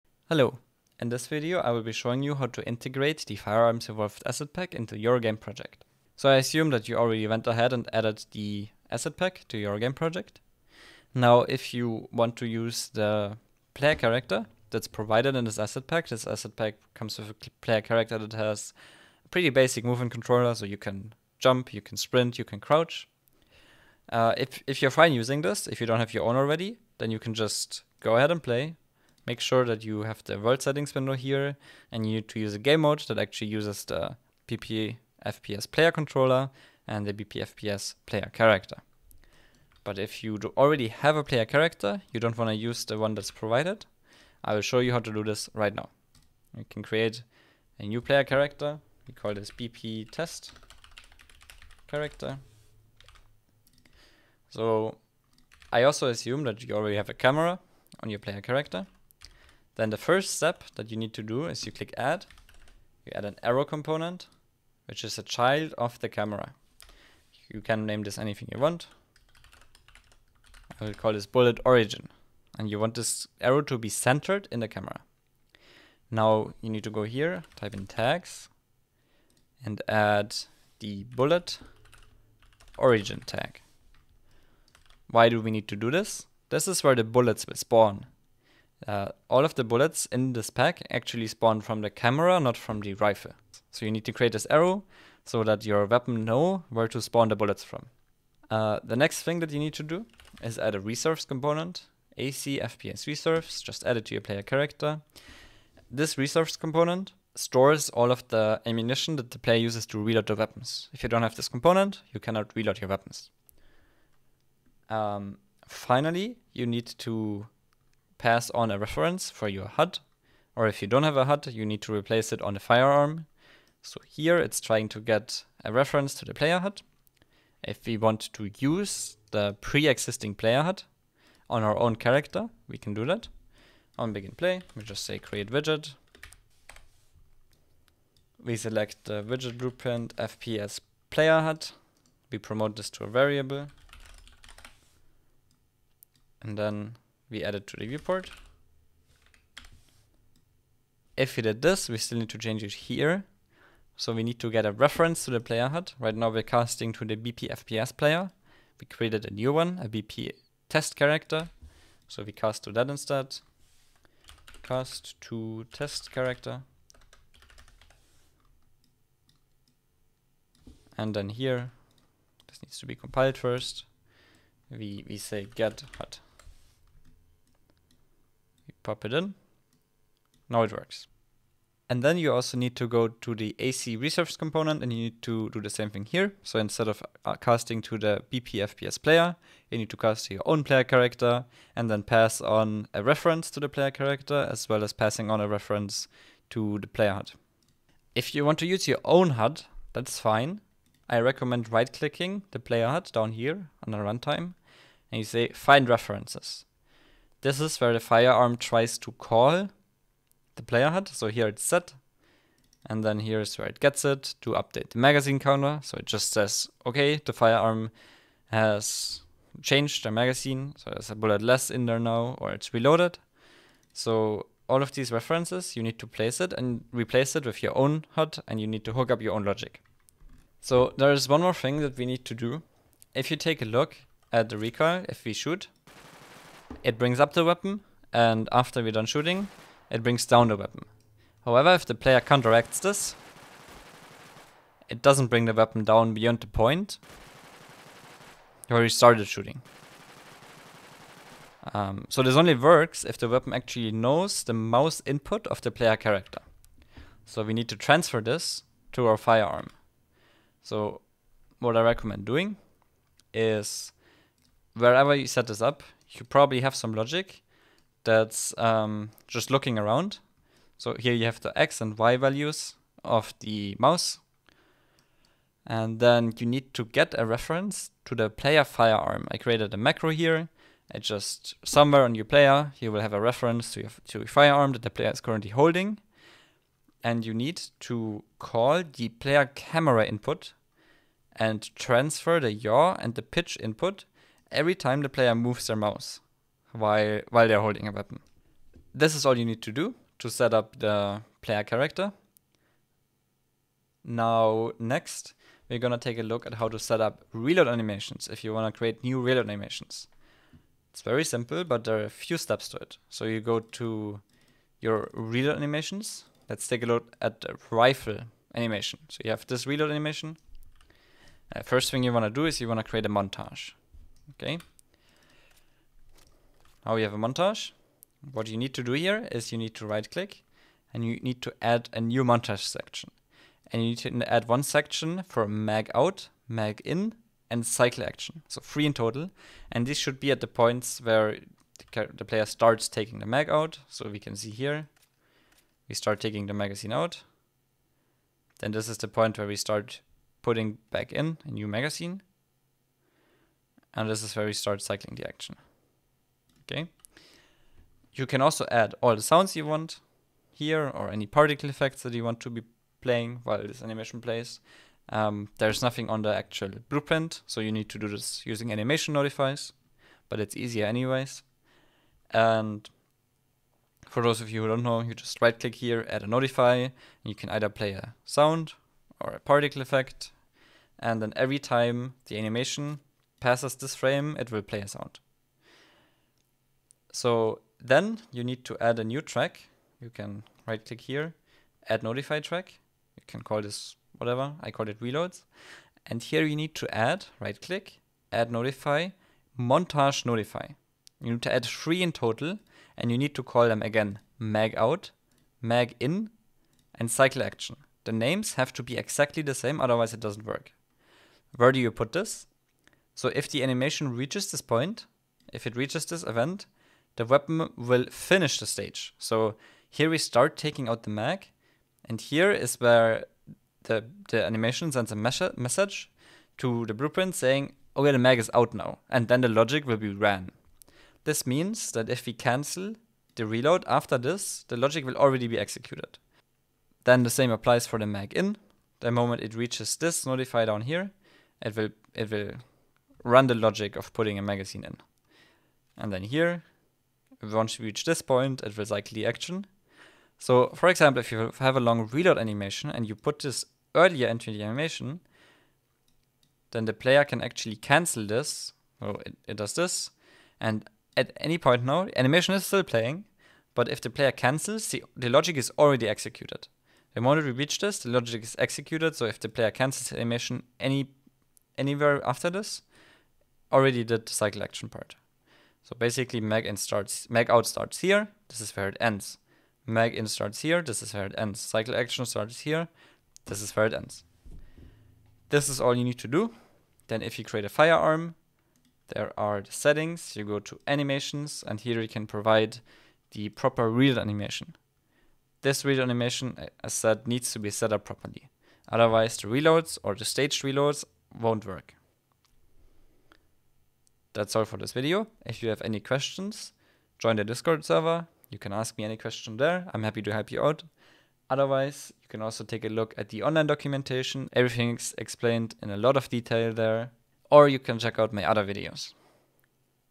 Firearms Evolved - Quick Start Guide
This video demonstrates how to integrate the Firearms Evolved pack into your game project.

00:00 - Introduction
00:45 - BulletOrigin Arrow
01:40 - Reserves Component
02:00 - Player HUD
03:50 - Player Camera Input
05:03 - Reload Animations
08:24 - Outro